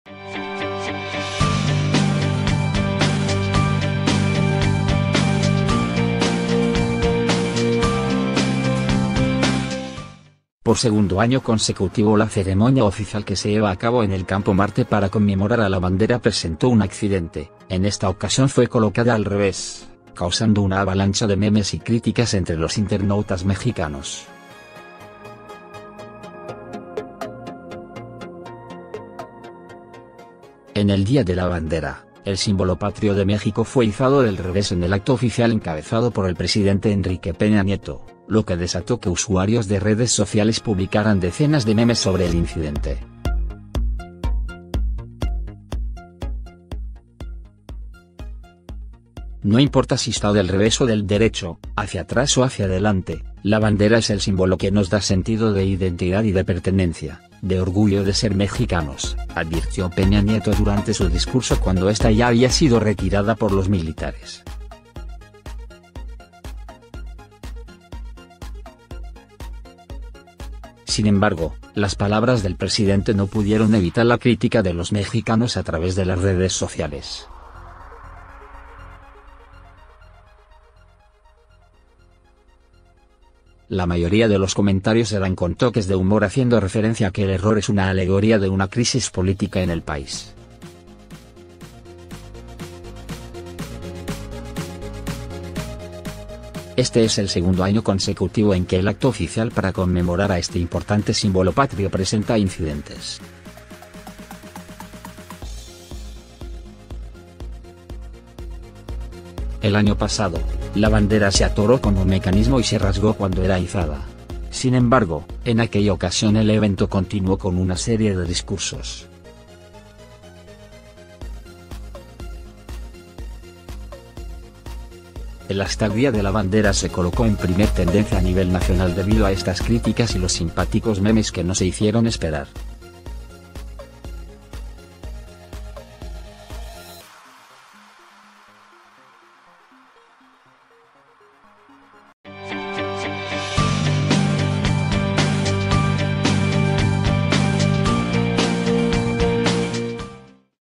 EPN aparece de cabeza en los MEMES tras el error de izar la bandera con el escudo al revés
Por segundo año consecutivo la ceremonia oficial que se lleva a cabo en el Campo Marte para conmemorar a la bandera presentó un accidente, en esta ocasión fue colocada al revés, causando una avalancha de Memes y críticas entre los internautas mexicanos.

En el Día de la Bandera, el símbolo patrio de México fue izado del revés en el acto oficial encabezado por el Presidente Enrique Peña Nieto, lo que desató que usuarios de redes sociales publicaran decenas de memes sobre el incidente.

Fuente: goo.gl/sWMp2x